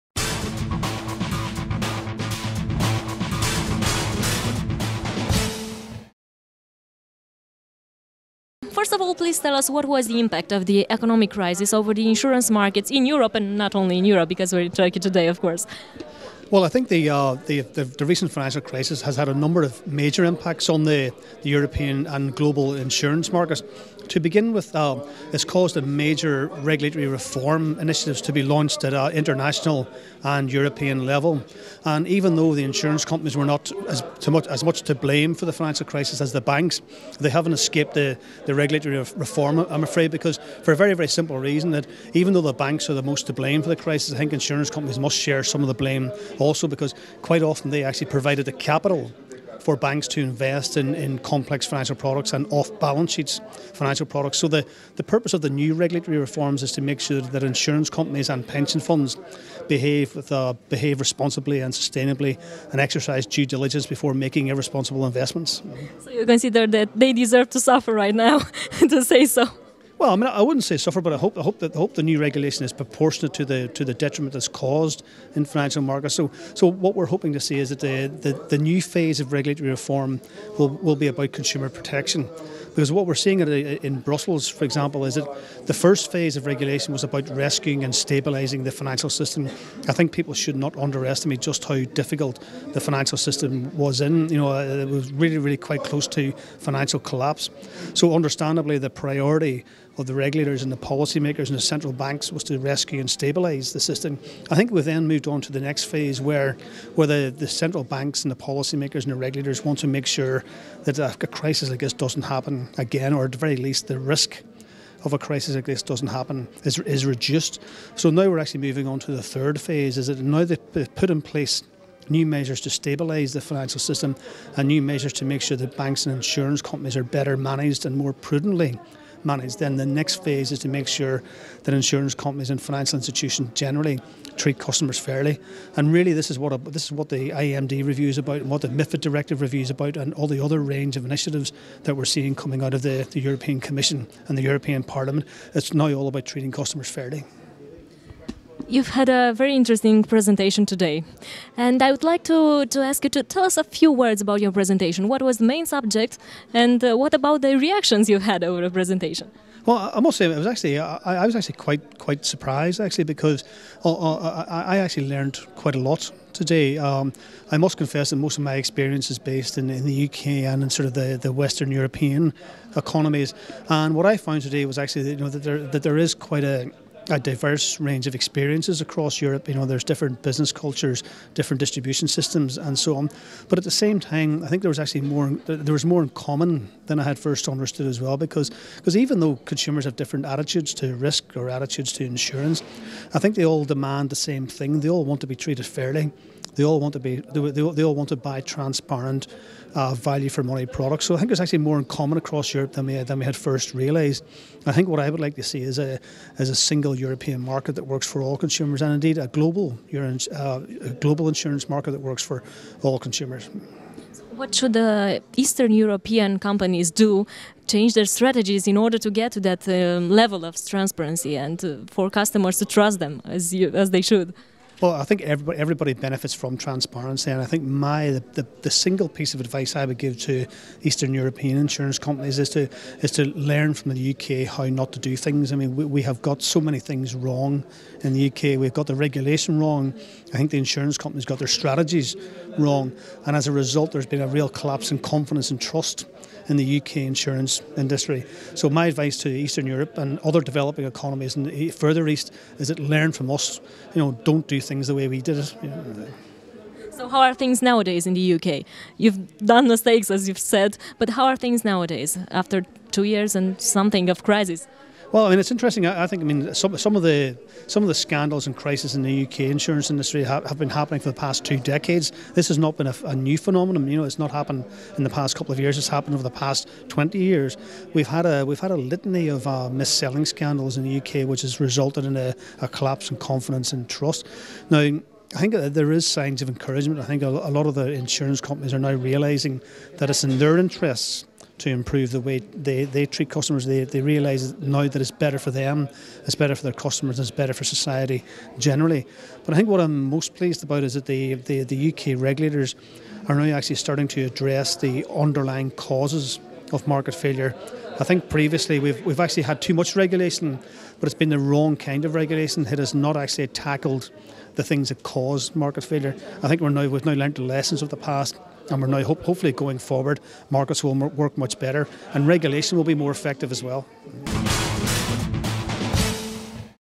Mick McATEER Director, The Financial Inclusion Center
"Conferinta Internationala de Intermediere in Asigurari"
Editia I

28-29 martie, Istanbul

Interviu realizat de
Oana RADU
Florin TURICA